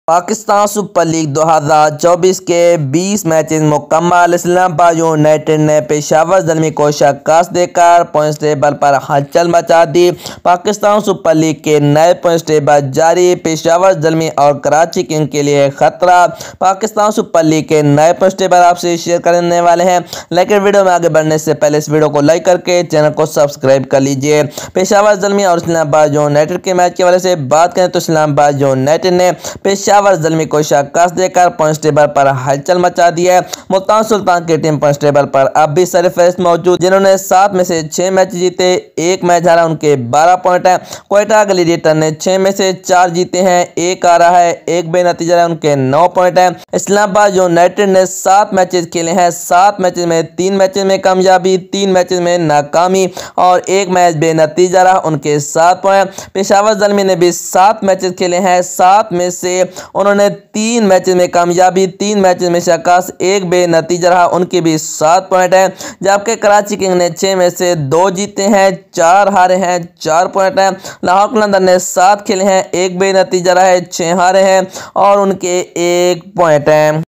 PSL points 2024 | Peshawar Karachi in danger | Multan Sultans Qualify for playoffs
🌎🌎. ||| PlayList Videos |||. 🌎🌎

🔶🔶. ||| Social Links |||. 🔶🔶